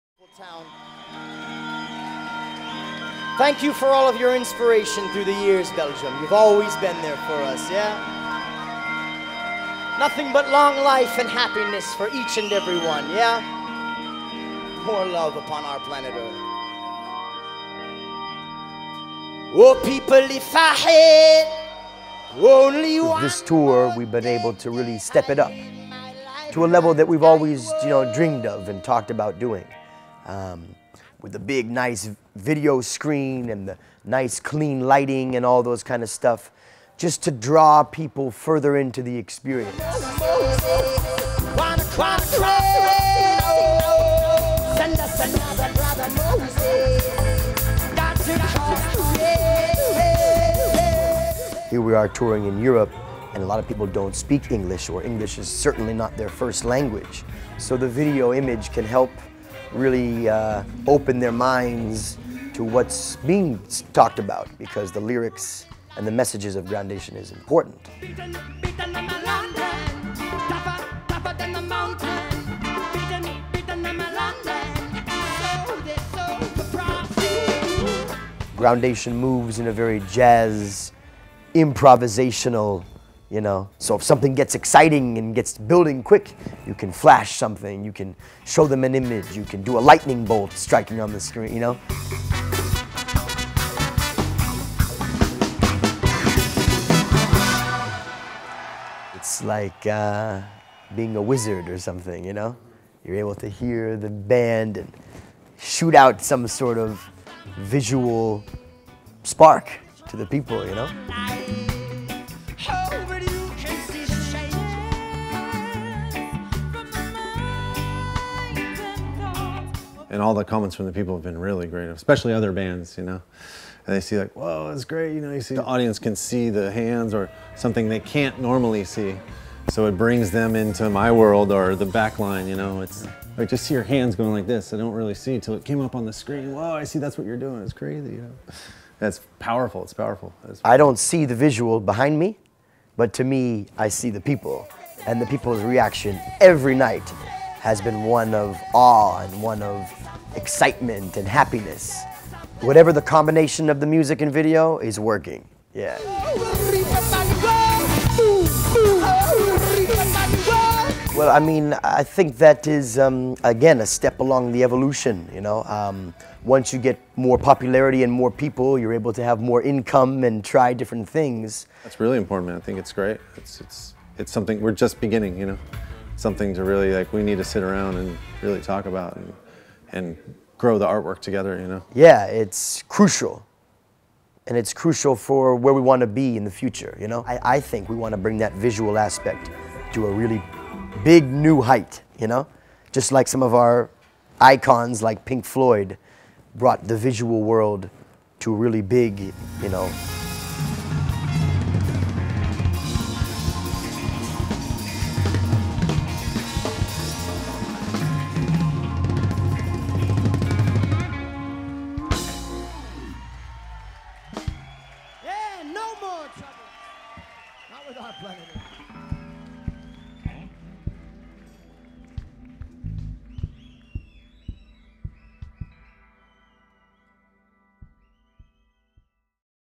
Groundation Live... Music.. and Video !
Groundation en Live avec maintenant de la vidéo ! A l'occasion du Bob Marley Tour 2011, Groundation a mis en place une scénographie vidéo originale. Le VJing évolue en fonction de la musique, de l'improvisation pour que chaque show soit unique. Groundation et les visuels en Live, c'est une aventure qui continuera lors de la prochaine tournée d'automne.

Lumières : Luc Kerouanton
Visuels : VJ Stello